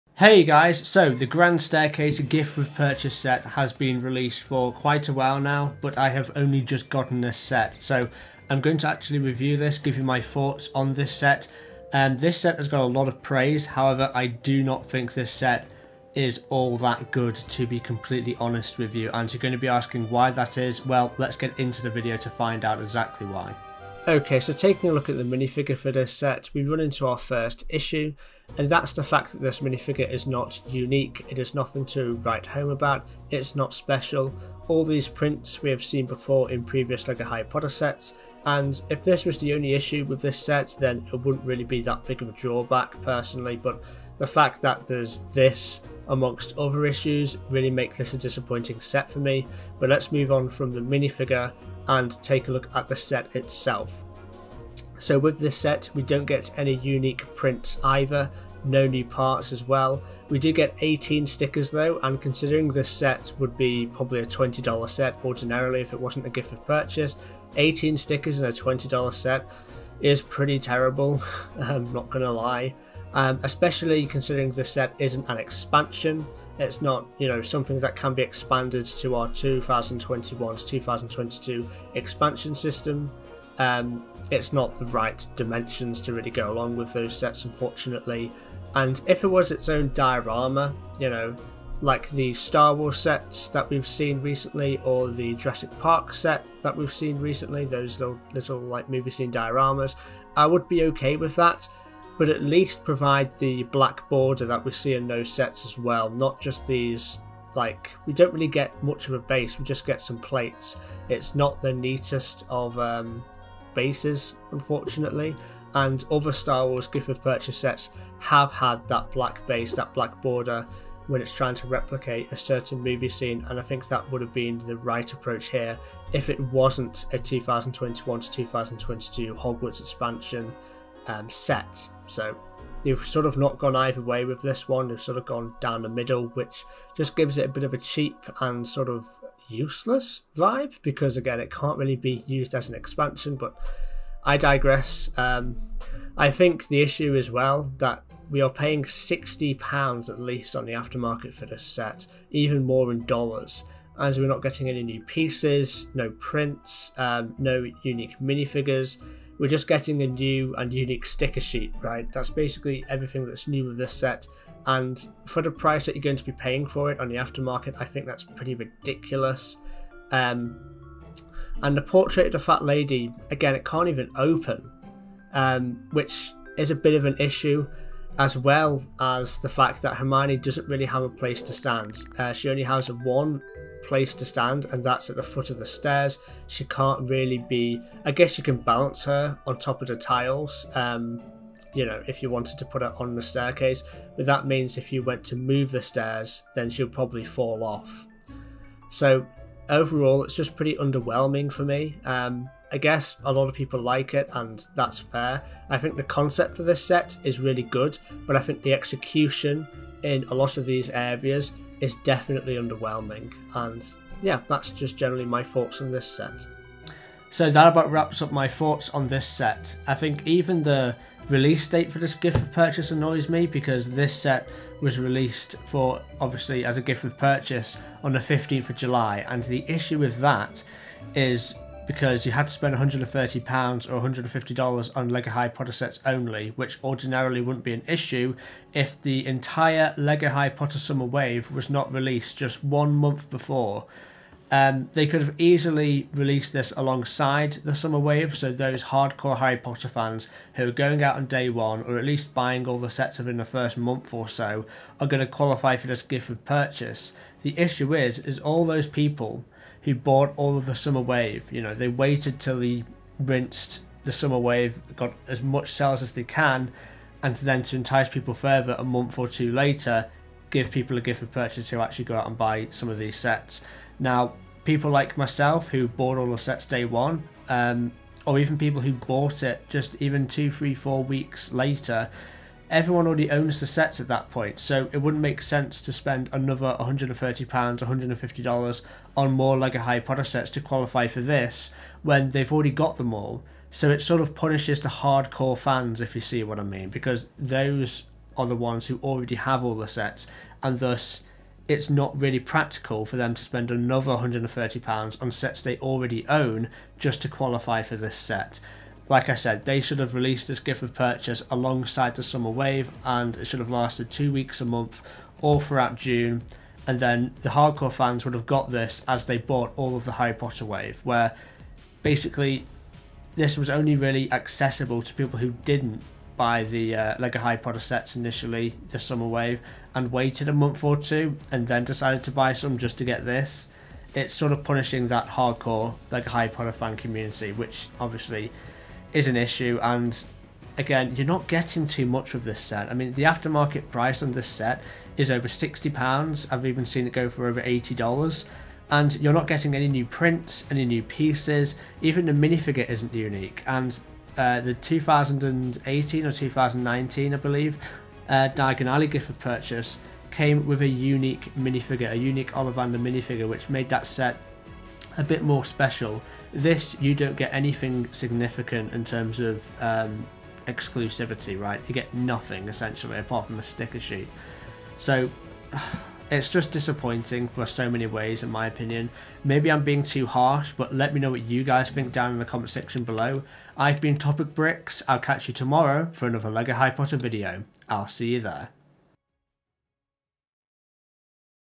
DON'T BUY THIS LEGO HARRY POTTER SET!
Welcome to my hundred and ninety-ninth YouTube video where I review the disappointing 2022 LEGO Harry Potter Grand Staircase Gift With Purchase set!

Become a MEMBER of the TopicBricks community...

Socials...

DOWNLOAD Instructions HERE!

Play Harry Potter: Hogwarts Mystery HERE!